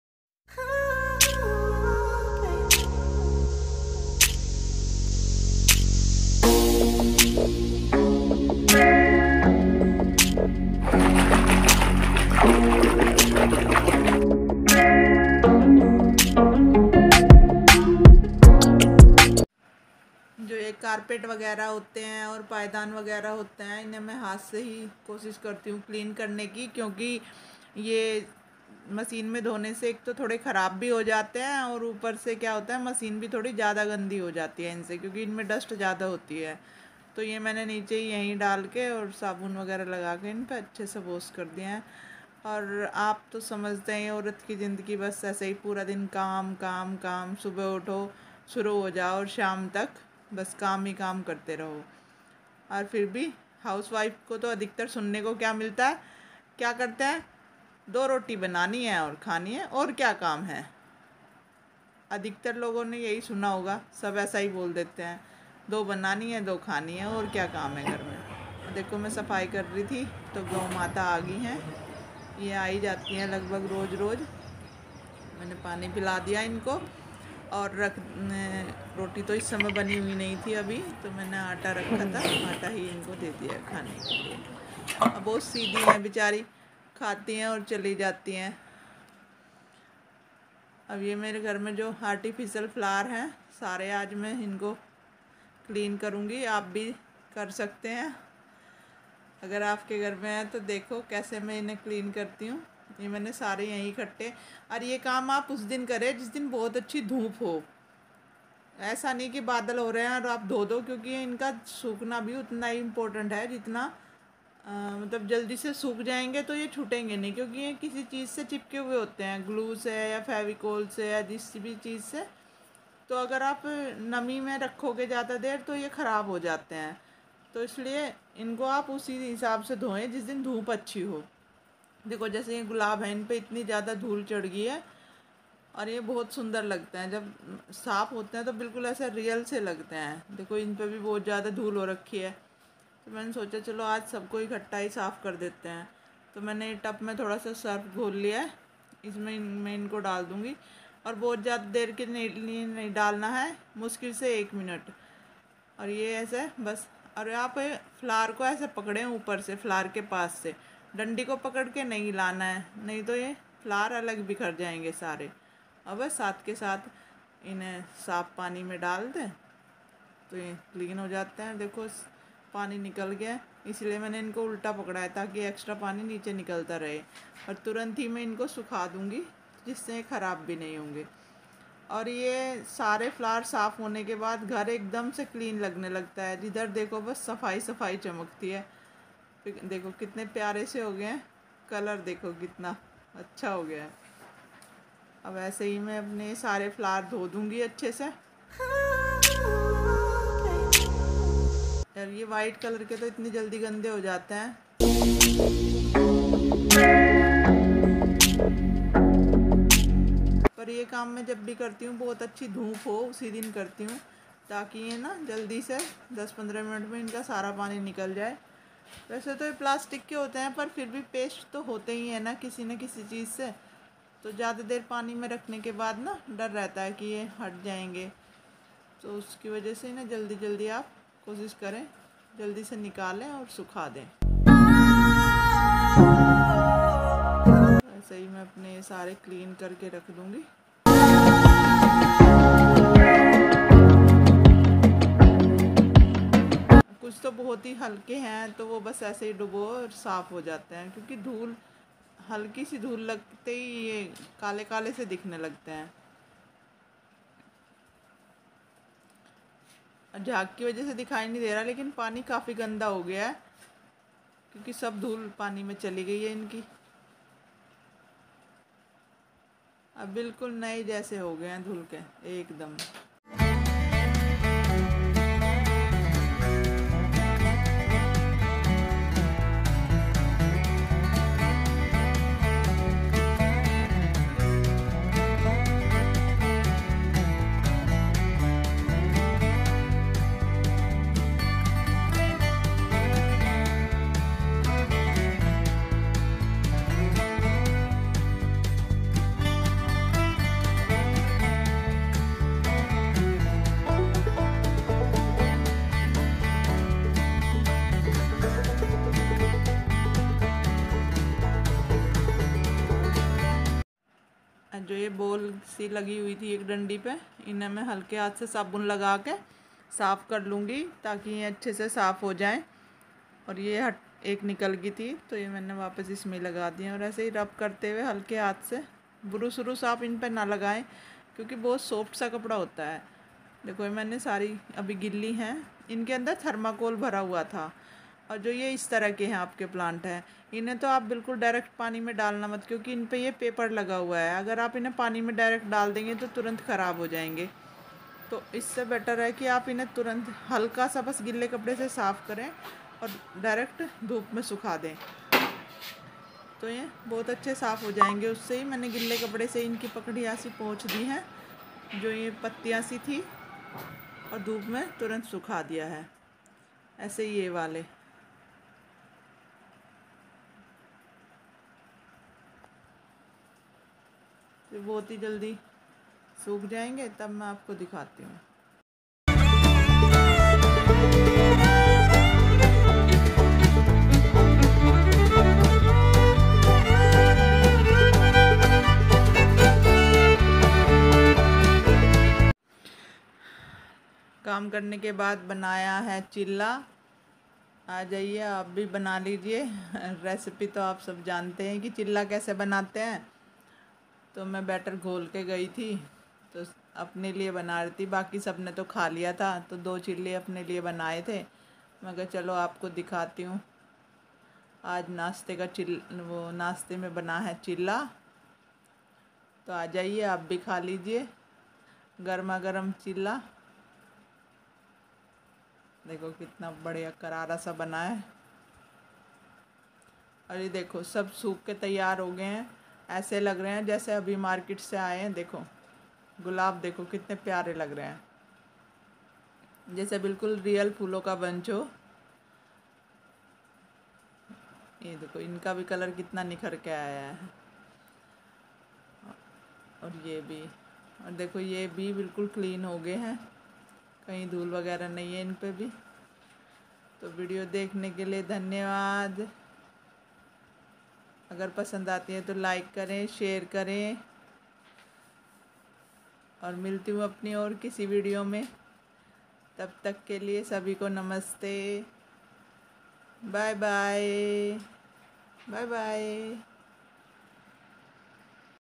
House wife ki life pura din kam kro aor fir tane suno, kya kam h,दो पोनी ,दो खानी ओर क्या करती हो
राधा होम ट्रीक, Radha home trick,